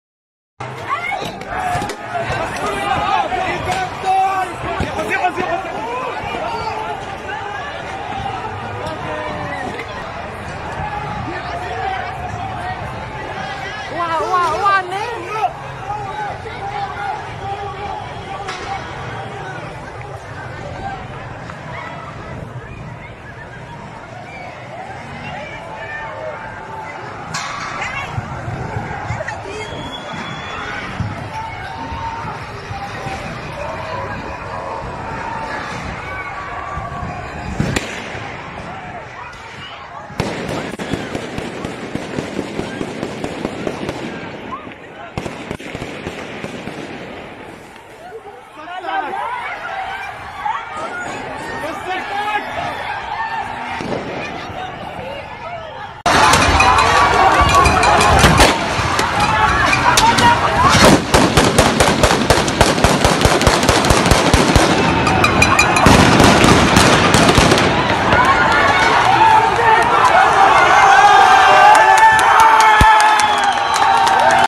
الموقع \فيديو ثاني للموكب الذي اقتحم المتظاهرين امام مجلس النواب
فيديو ثاني للموكب الذي اقتحم المتظاهرين امام مجلس النواب\ متابعة حسن الخواجة